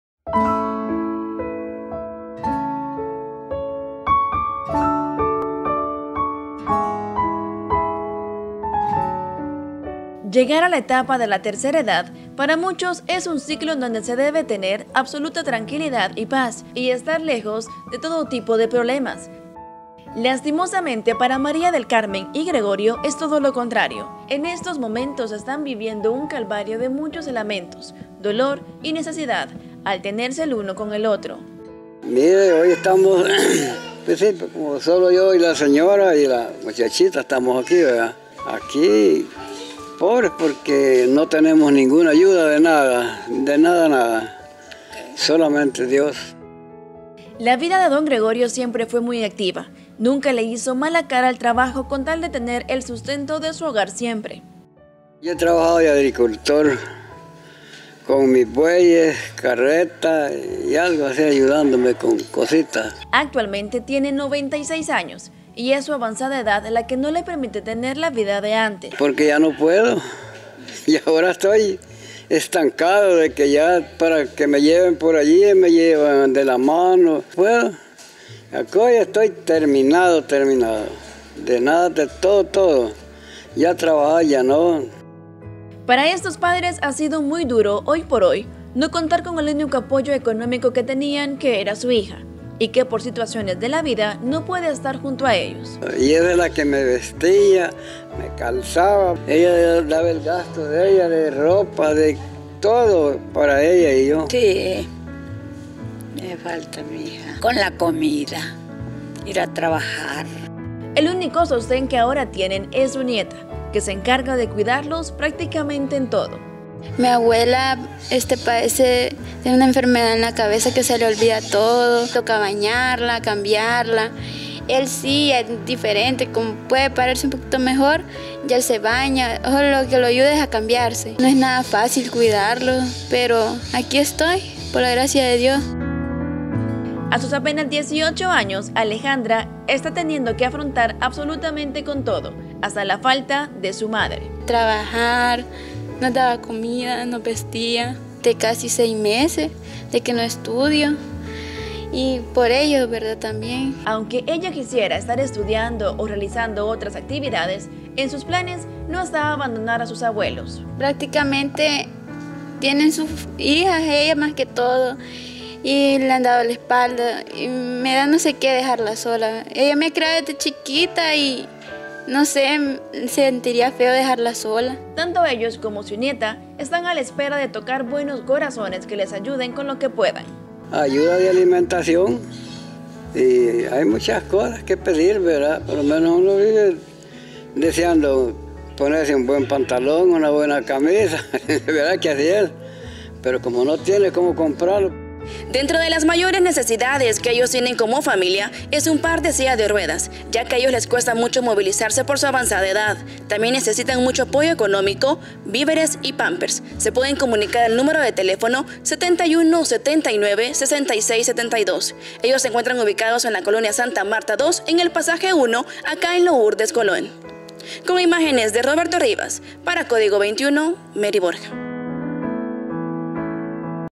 🤲🏽🕊️Niña María y Don Gregorio en desamparo👴🏽👵🏾
En Lourdes Colón, encontramos a esta pareja de ancianitos, dos personas de avanzada edad, que toda su fueron trabajadores, pero por motivos ajenos, ahora se encuentran solos. Teléfono de ayuda: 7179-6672